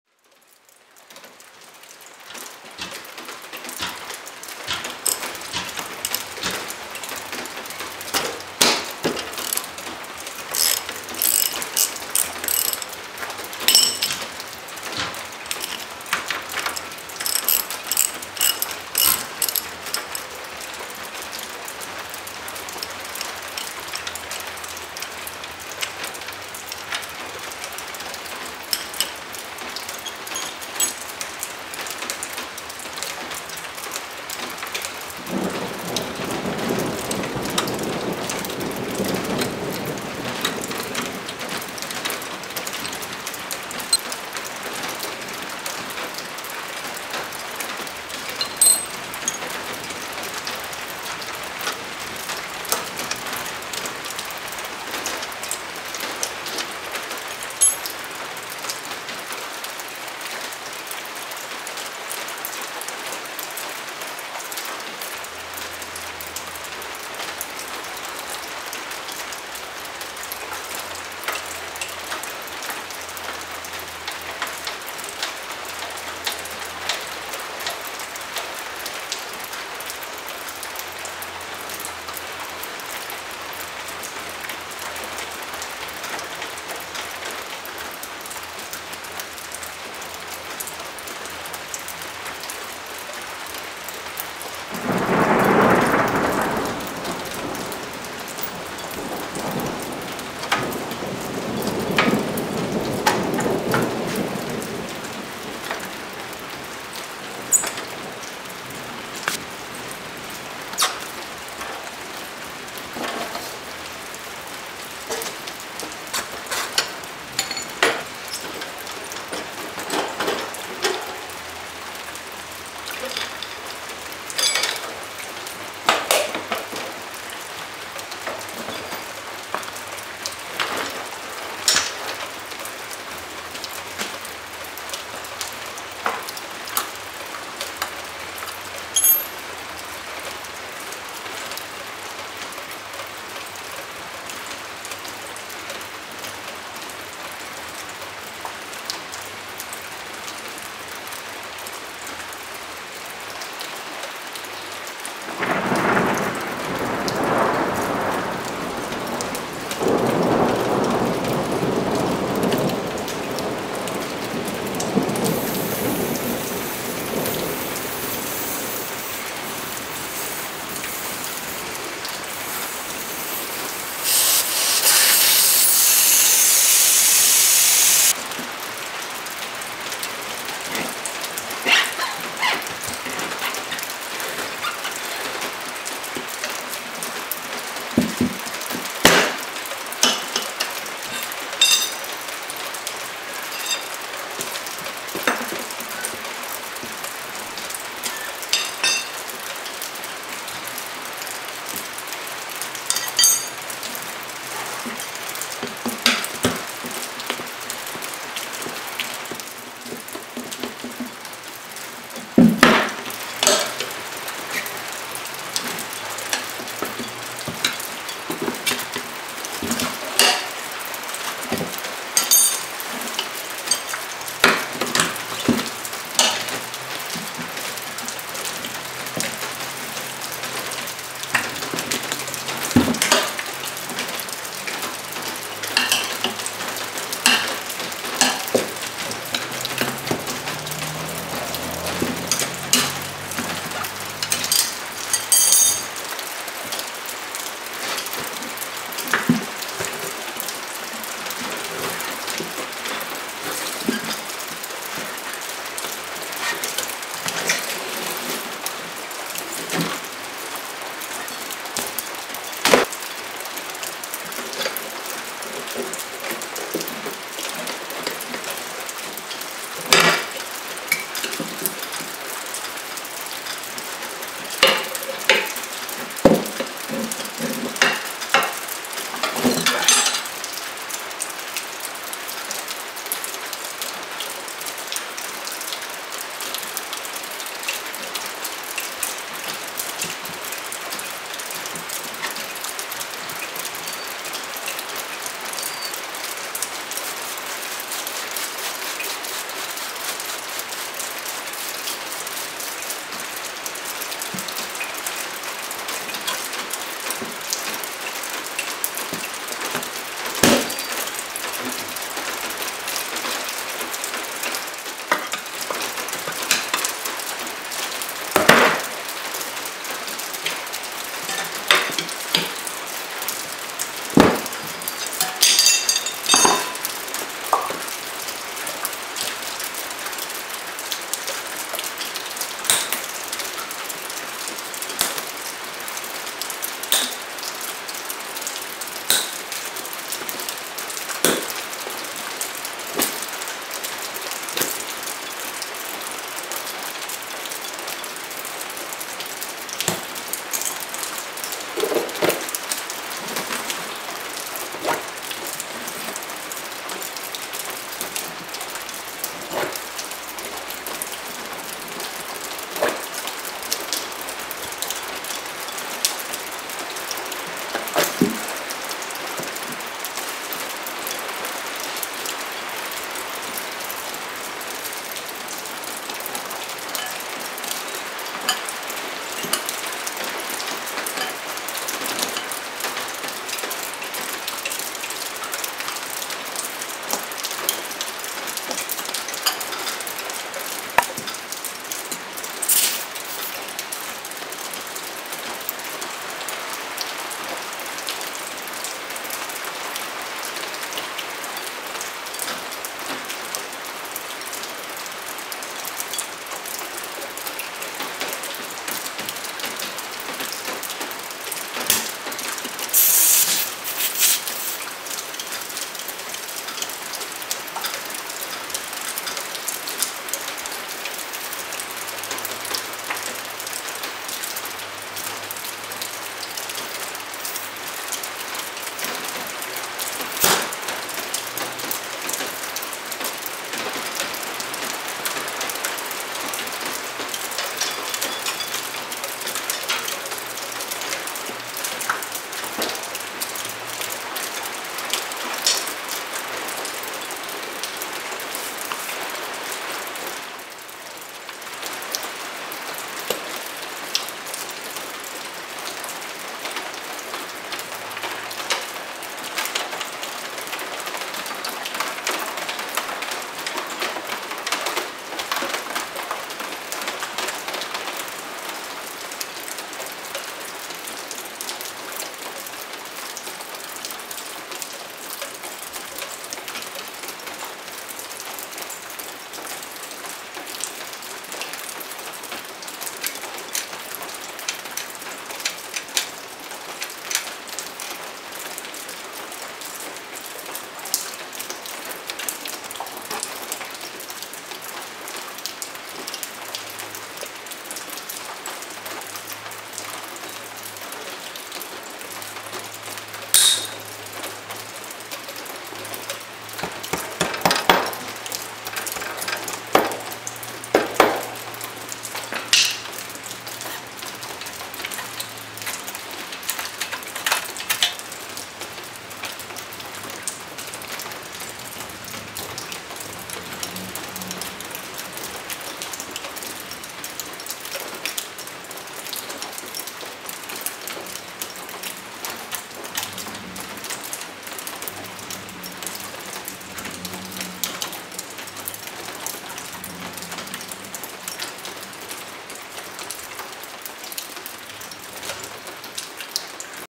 How to change a dirt bike tire, quietly.
After my feeble attempts at hill climbs on a nearly bald tire, I was feeling mello but into a tire swap. The following video is of what ensued.